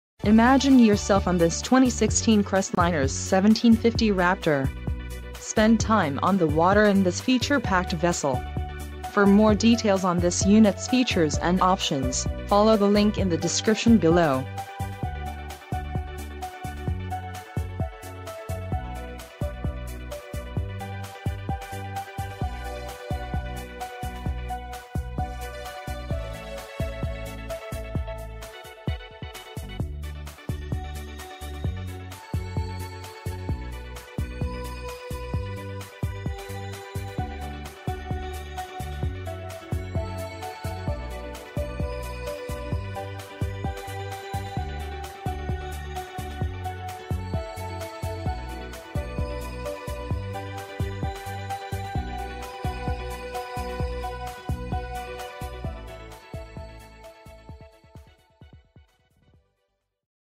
2016 Crestliner 1750 Raptor - Huntsville Marine - Huntsv...
For Sale in Huntsville, ON P1H 1B5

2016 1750 RAPTOR.  Summit White with Black Metallic Accent and Tan Interior. Vinyl Floor, Cockpit and Decks. Mercury 115 4S 13x18 Vengeance Prop.  And much more... Max HP: 150 hp Length Overall: 17 ft. 8 in. Beam: 96 in. Dry Weight: 1,870 lb. Max Person Capacity: 6 persons Fuel Capacity: 40 gal. Max Carry Load: 1,600 lb.